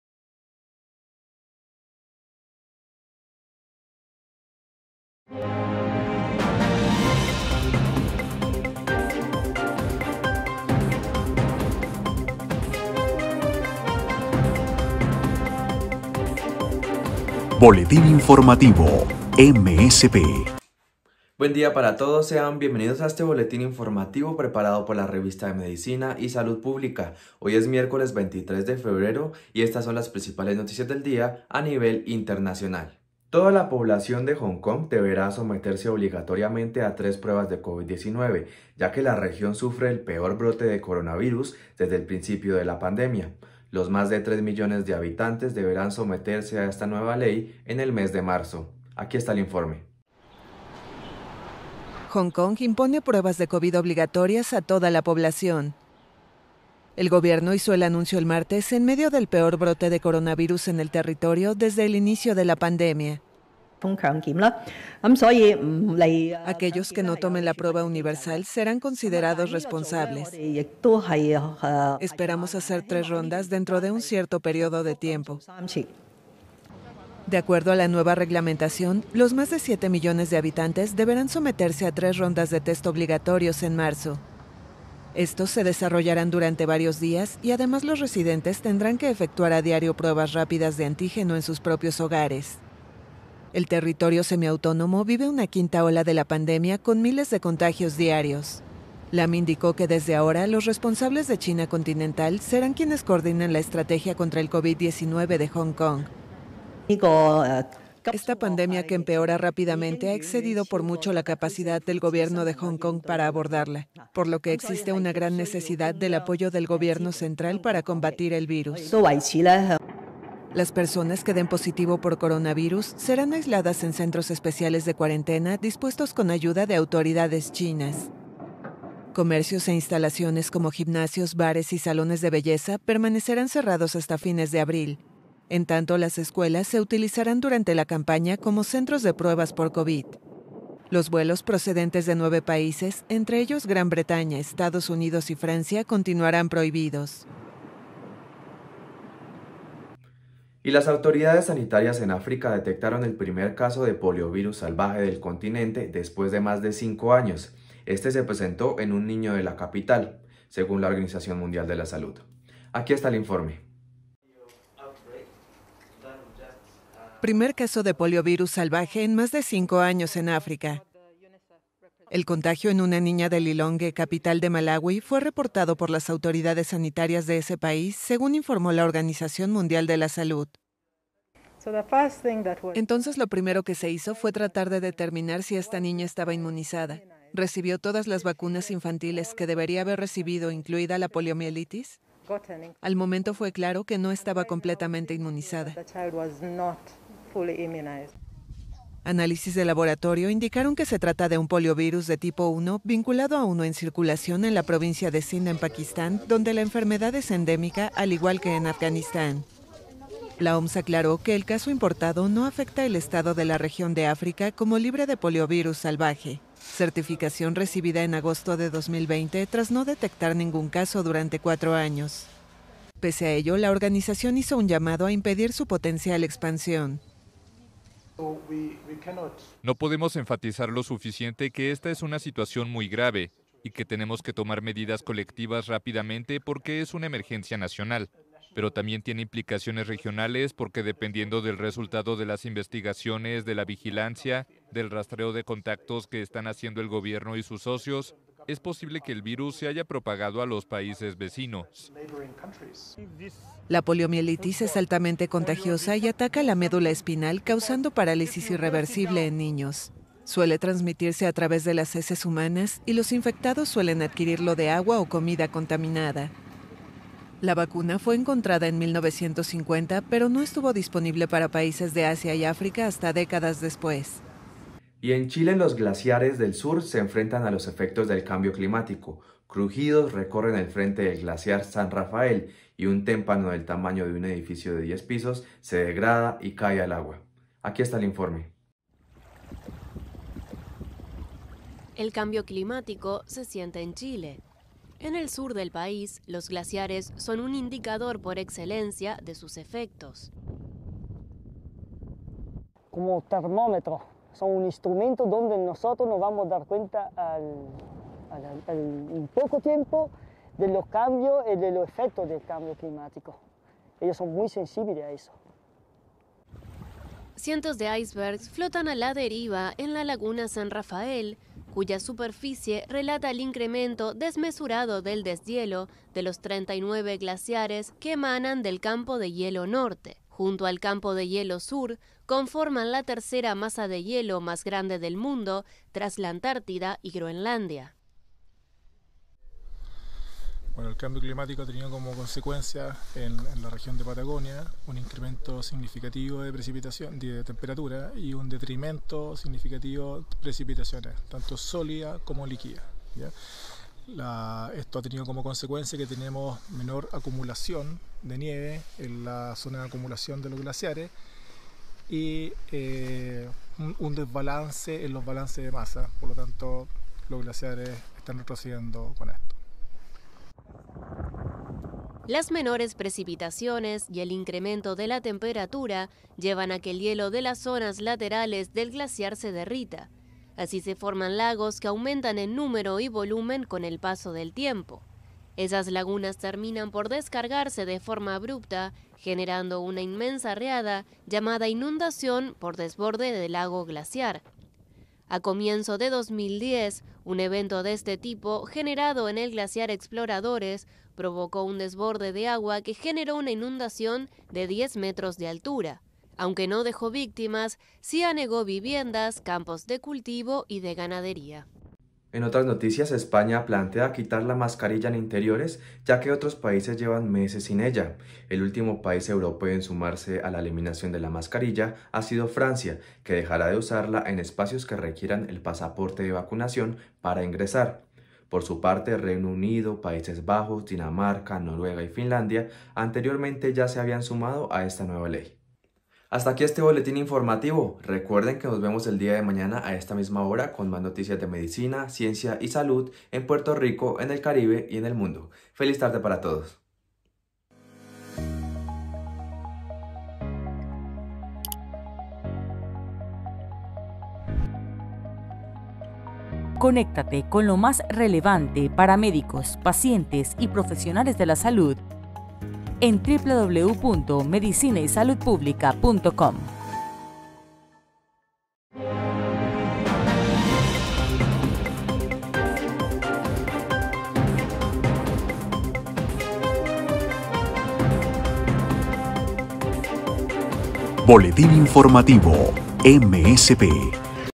Población de Hong Kong deberá someterse obligatoriamente a pruebas de Covid-19
EnVivo I Autoridades sanitarias en África detectaron el primer caso de poliovirus salvaje del continente.

-En Chile los glaciares del sur, se enfrentan a los efectos del cambio climático.

-España plantea quitar la mascarilla en interiores.